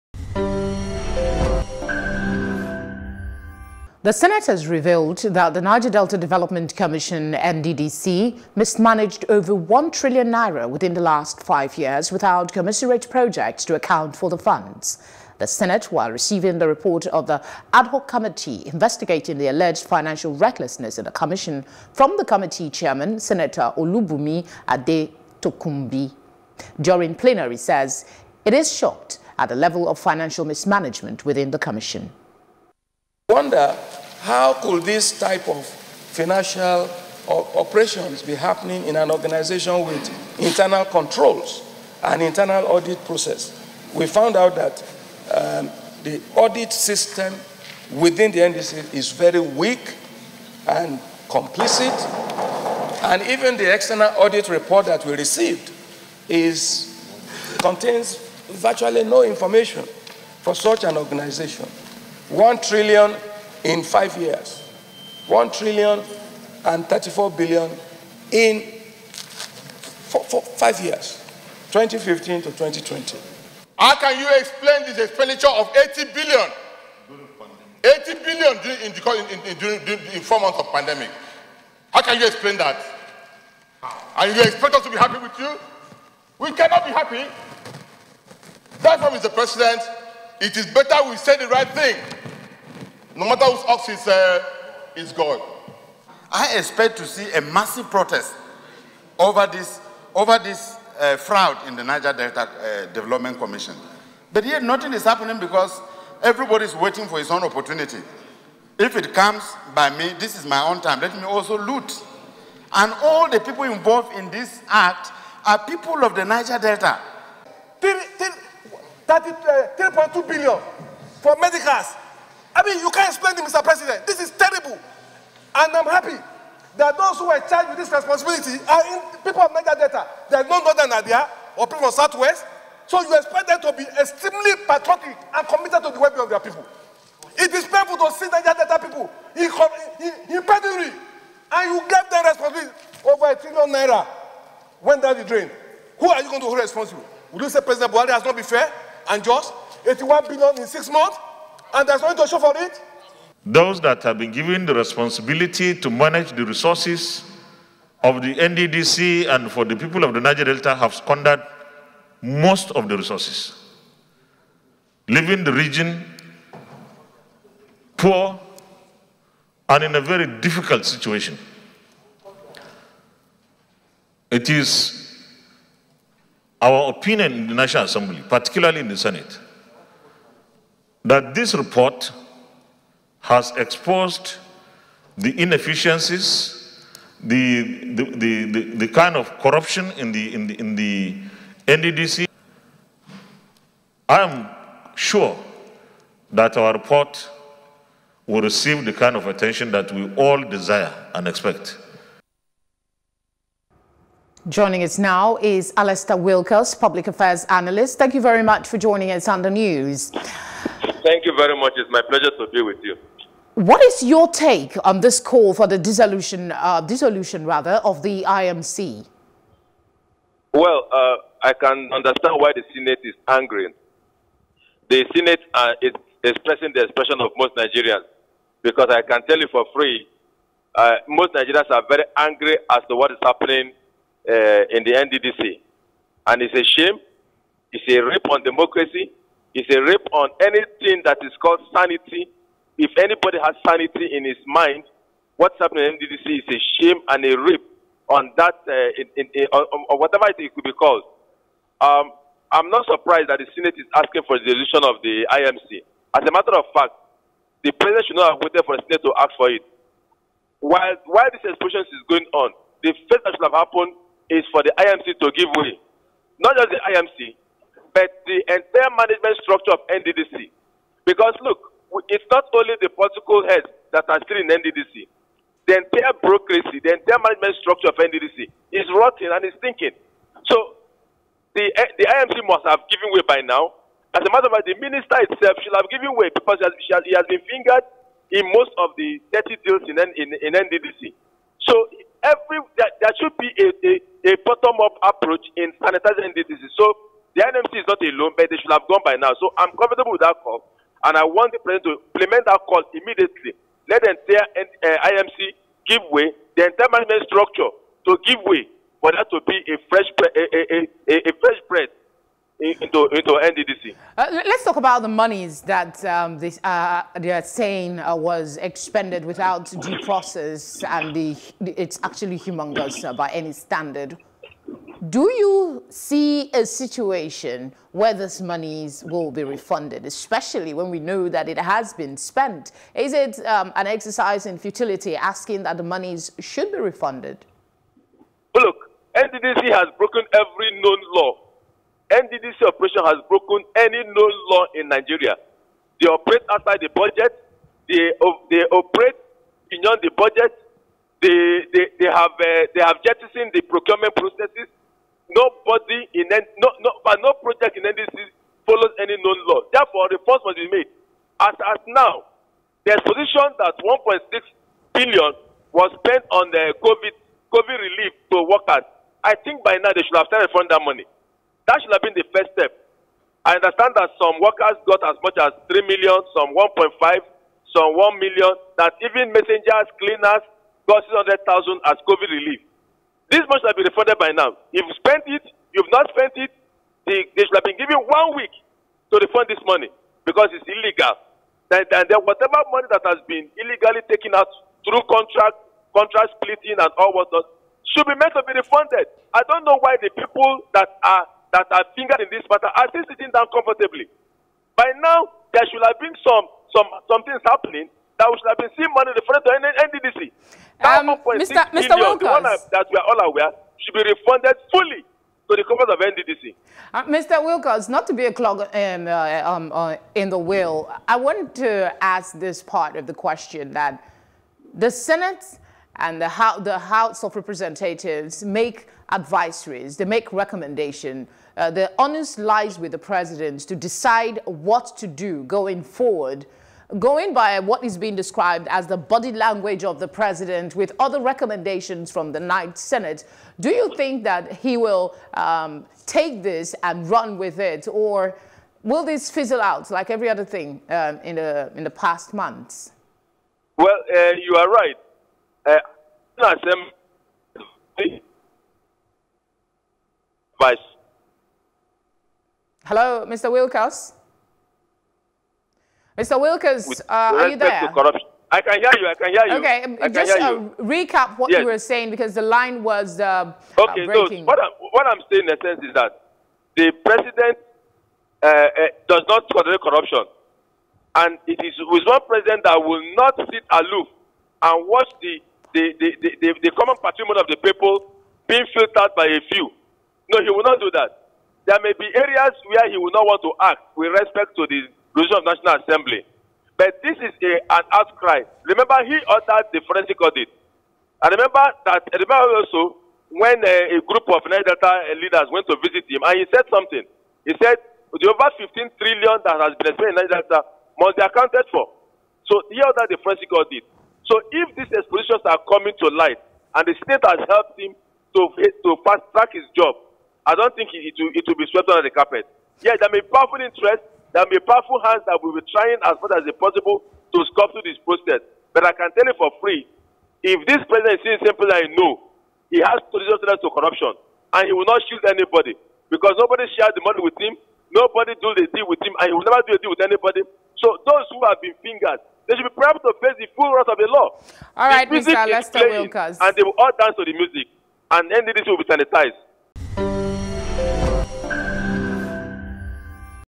NDDCProbe: Senate Announced That NDDC Mismanaged Over N1Trillion and N34Billion in 5 Years
NDDC Probe Fallout Senate Panel Recommends Dissolution Of IMC